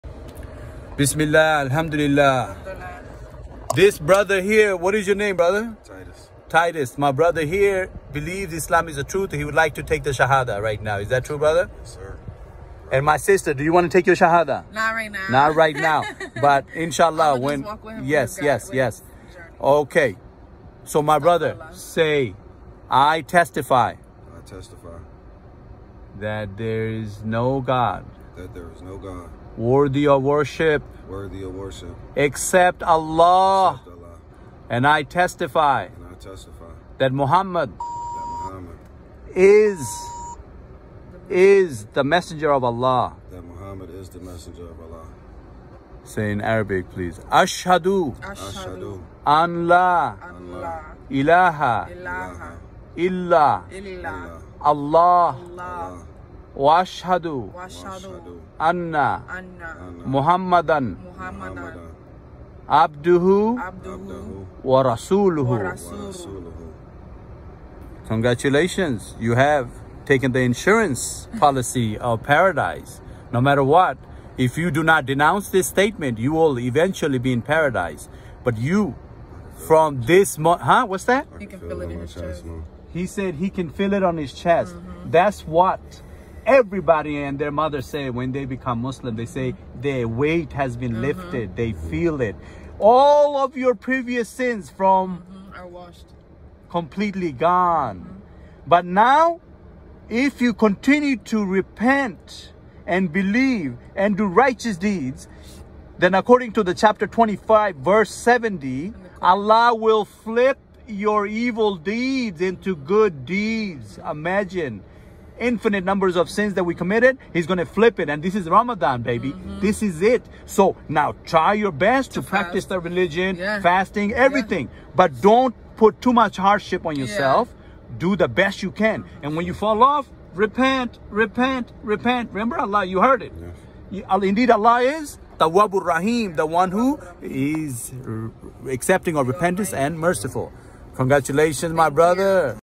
Taxi sahada, he can “feel it in his chest”
All people need is to hear the Quran. And they will feel it in their chest. Alhamdullillah!!!